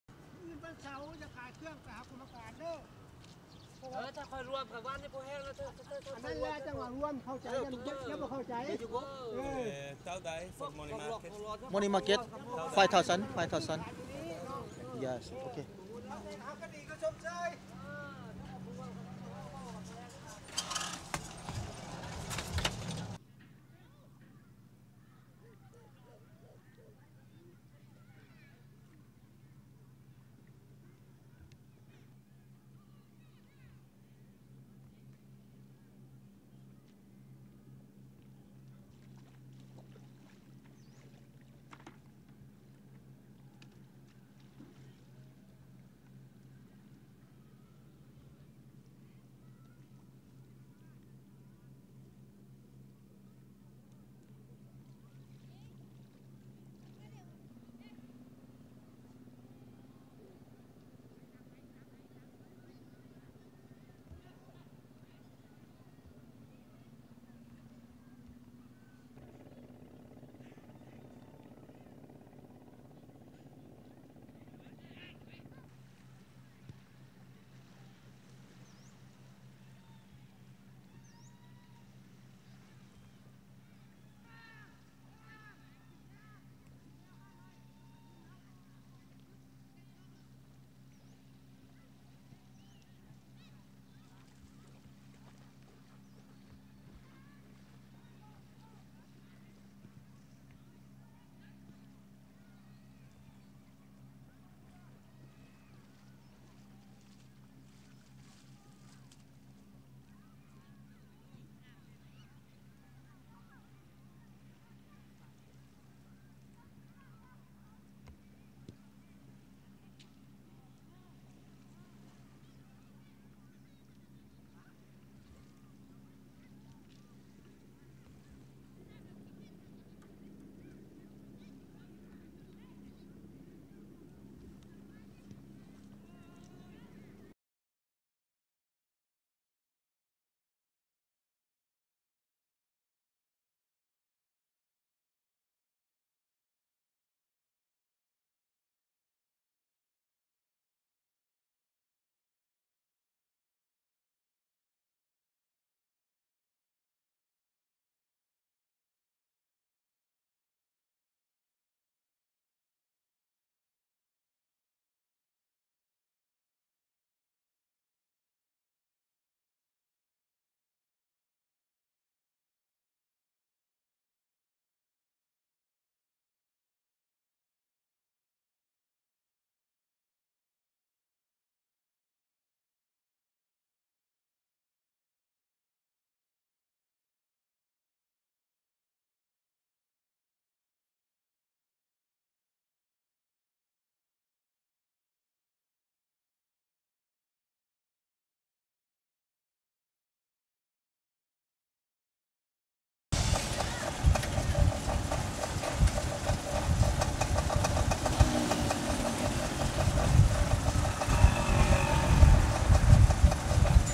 Mekong Planet
Tratto da Laos Planet by Cicoriello .Le rive del Mekong in Laos. Luogo incantato. Come taxi si prende il tuc tuc.